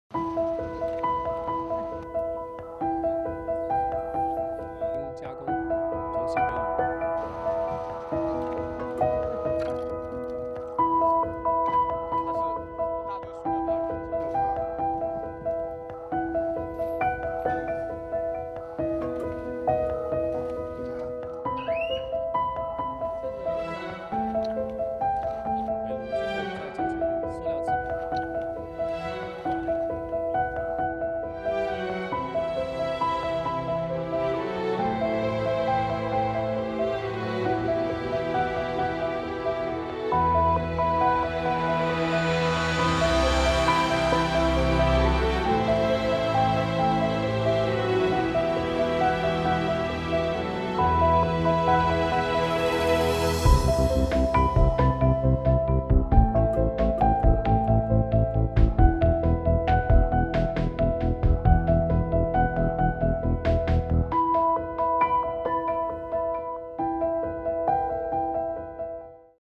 Chinese garbage smugglers arrested after 5,000 tonnes of foreign waste crosses the border
Customs officials arrested 11 suspects that allegedly smuggled over 5,000 tonnes of foreign waste into China.